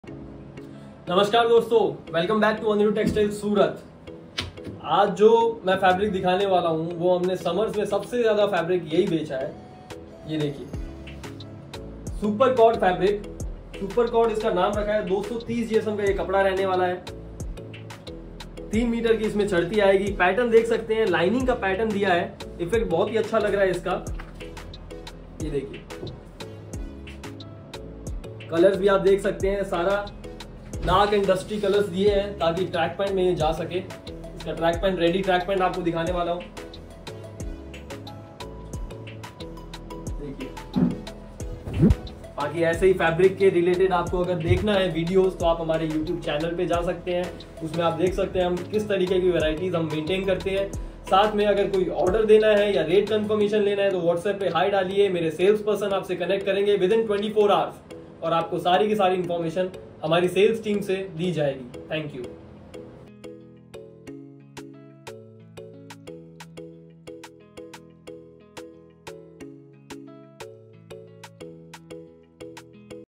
BARCODE FABRIC FOR TRACK PANT MAKING 9205293725
ANIRUDH TEXTILE, SURAT

QUALITY:- BARCODE
GSM :- 220 GSM
WIDTH :- 60” WIDTH
PREMIUM QUALITY FABRIC

SHOP ADDRESS :- 7022, SANGINI TRADE CENTER, saroli, surat